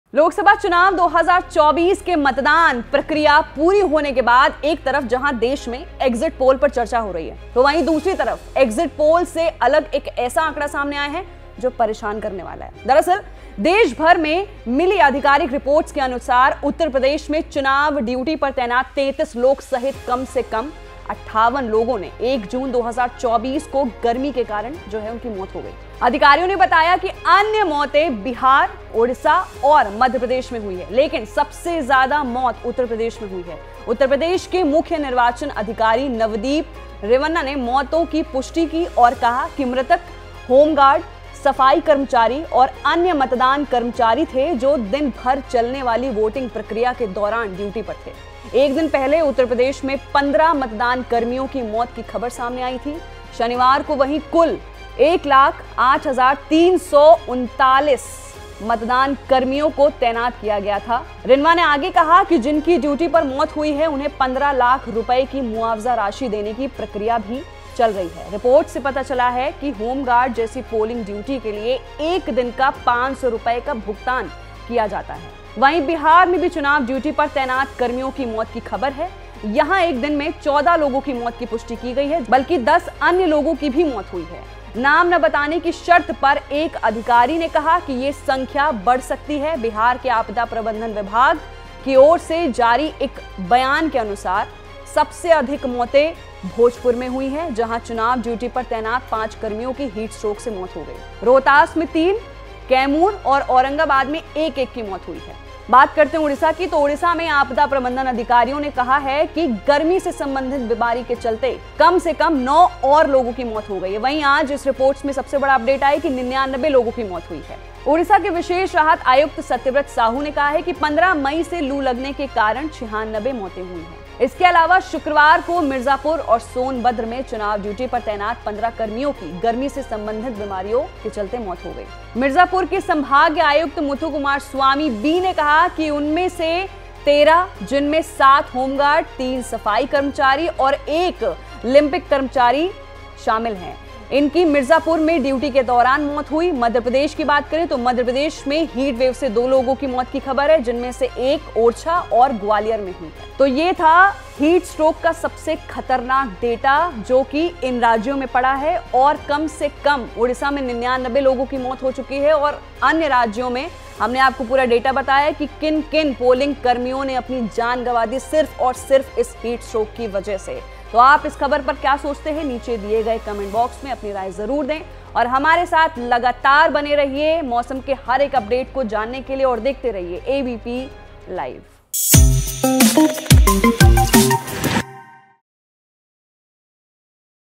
Elections Counting 2024: प्रचंड गर्मी ने पोलिंग कर्मियों को भी नहीं छोड़ा, 33 की हुई मौत |ABP LIVE
.लोकसभा चुनाव 2024 के मतदान प्रक्रिया पूरी होने के बाद एक तरफ जहां देश में एग्जिट पोल पर चर्चा हो रही है तो वहीं दूसरी तरफ एग्जिट पोल से अलग एक ऐसा आंकड़ा सामने आया है, जो परेशान करने वाला है. ...दरअसल, देशभर से मिली आधिकारिक रिपोर्टों के अनुसार, उत्तर प्रदेश में चुनाव ड्यूटी पर तैनात 33 लोगों सहित कम से कम 58 लोगों 1 जून 2024 को गर्मी के कारण मौत हो गई.